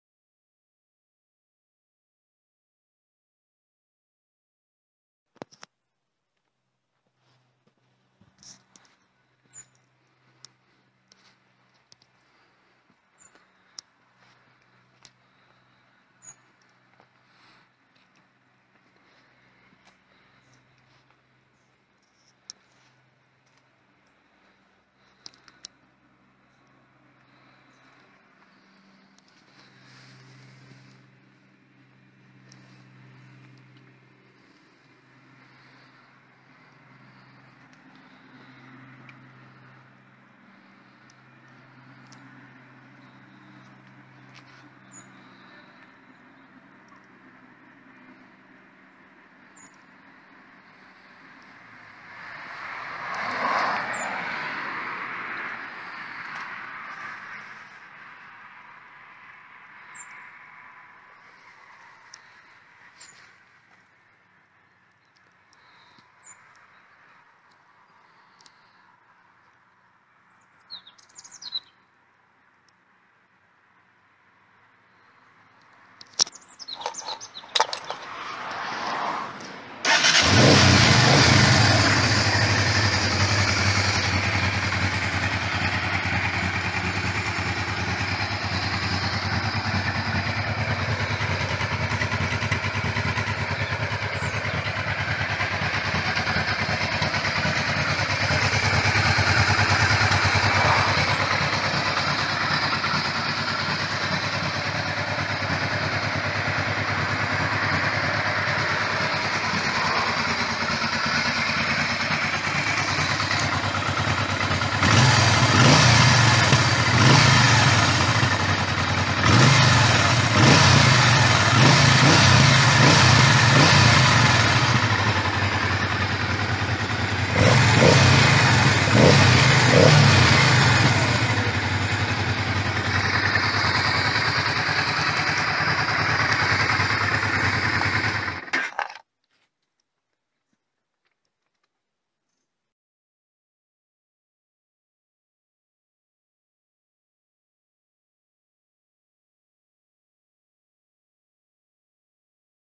My Dad's Motorcycle | Honda NTV650 Revere 1988 | Walkaround
This is a walk around of my dad's motorcycle, the Honda NTV650 Revere 1988.